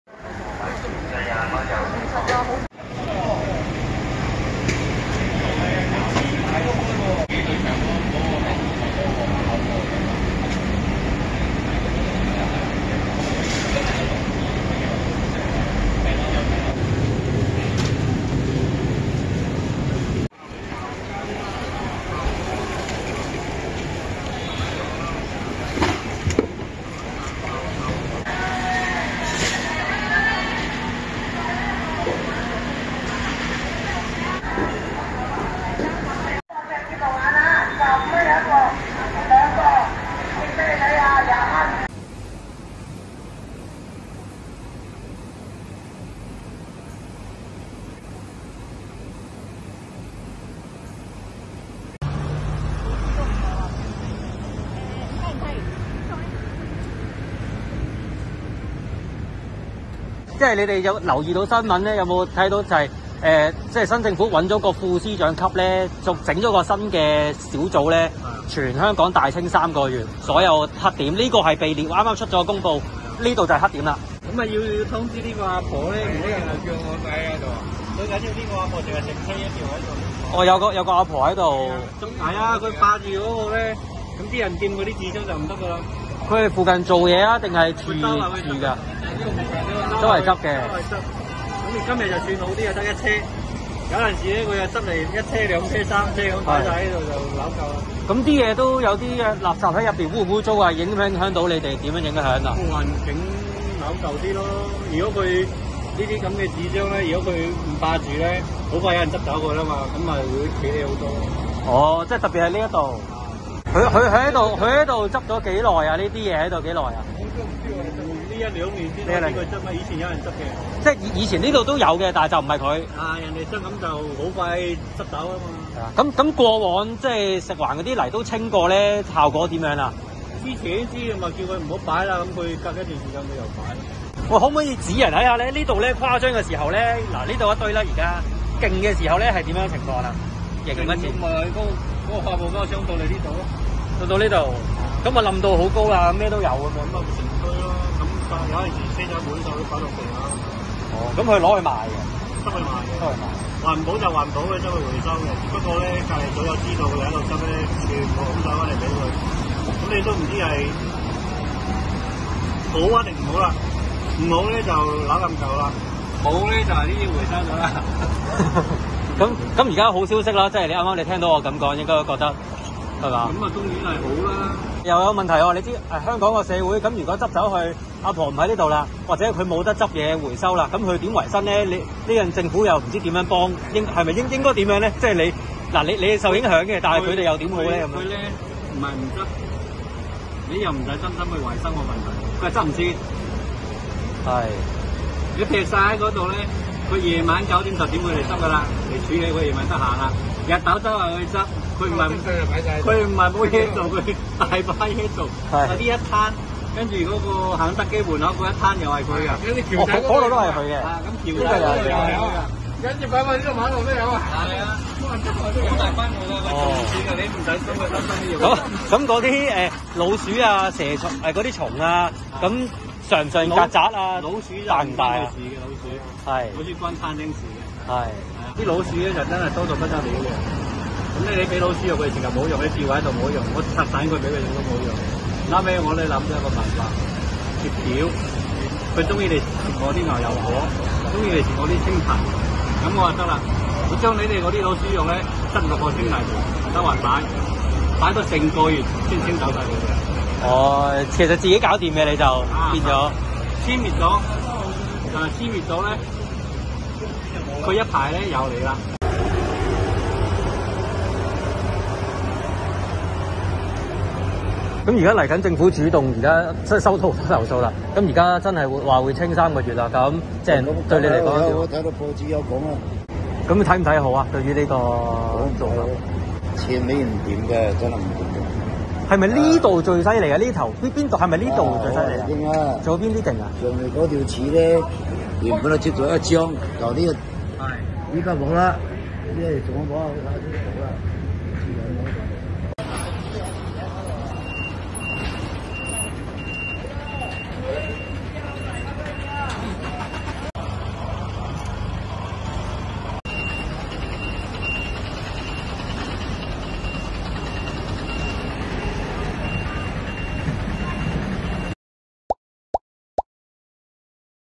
衛生黑點擾港多時 政府將打擊街道和後巷衛生問題
政府星期日起展開為期三個月的打擊衞生黑點行動，大公報記者近日到多區衞生黑點觀察，多條居民行經的橫街側巷，均堆滿垃圾或其他雜物，不少蒼蠅及蟑螂到處走，加上近日天雨濕漉，呈淺啡色的「垃圾水」流出，經過時嗅到陣陣異味，衞生環境十分惡劣。